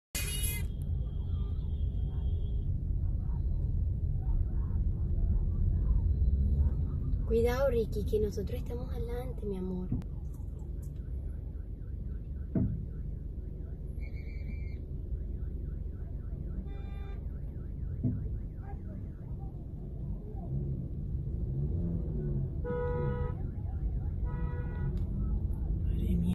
Habitante de la calle genera caos en vía a Cartagena
Somos ContrastesTV tu canal donde encontrarás información veraz, objetiva y actualizada local, nacional e internacional